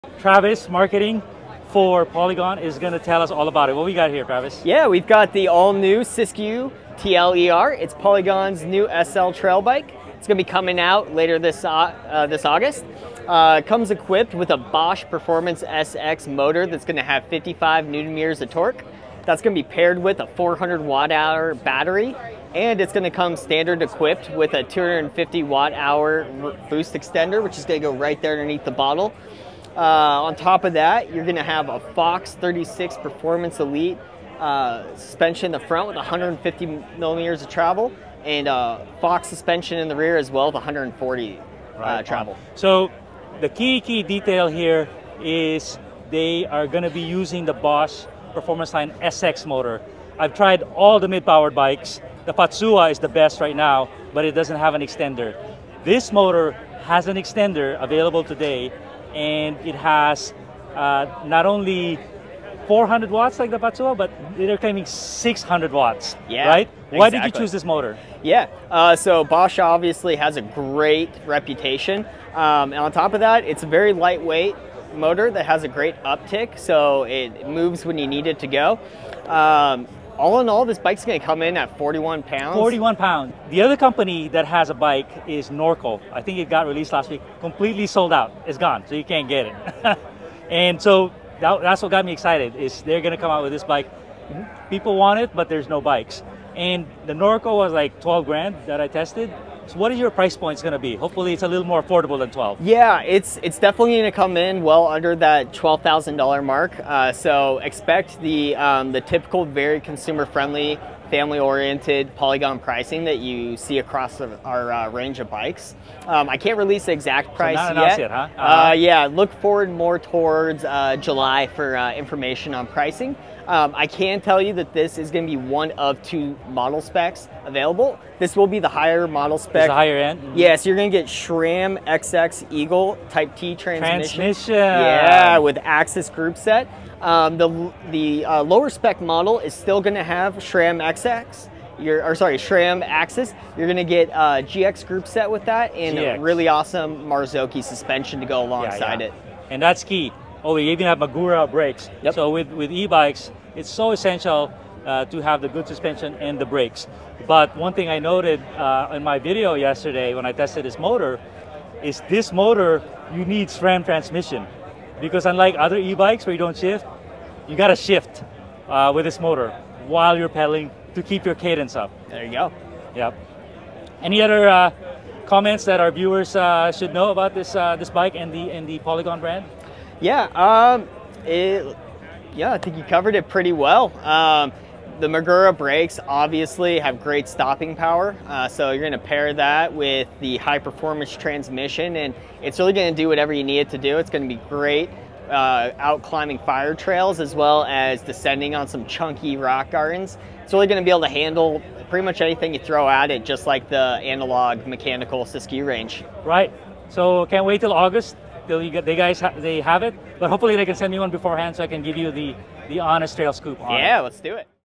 Polygon Siskiu TLE-R lightweight emtb with Bosch SX motor - Sea Otter Classic 2024
Mid-power emtb with budget-friendly pricing from Polygon. Equipped with the latest Bosch SX motor